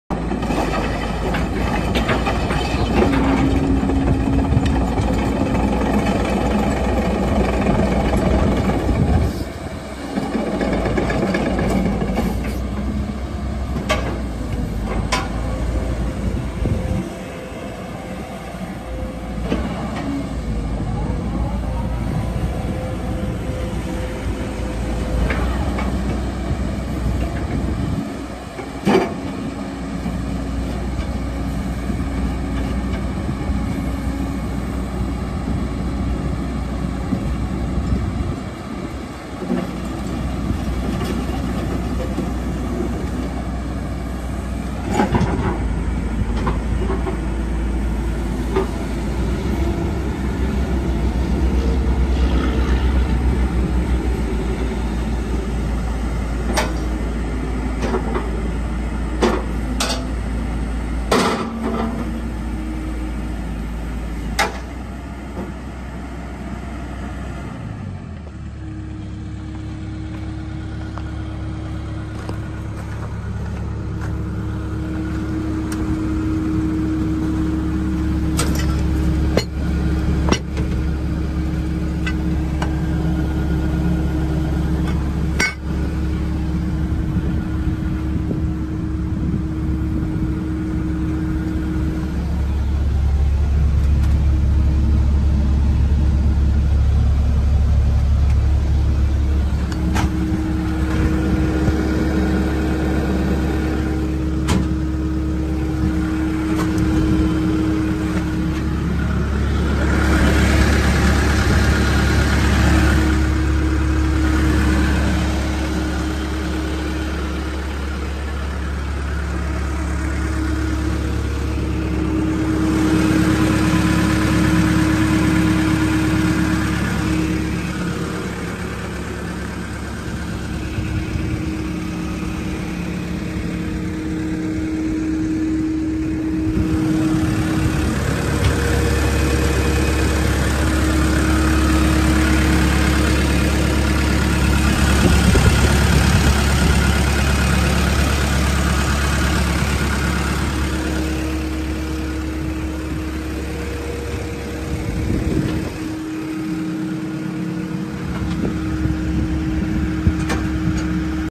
2020 CATERPILLAR 312GC For Sale
4 CYL CAT ENGINE, 20 IN PADS, 45 IN BUCKET, PATTEREN CHANGER, ENCLOSED CAB W/ A/C & HEAT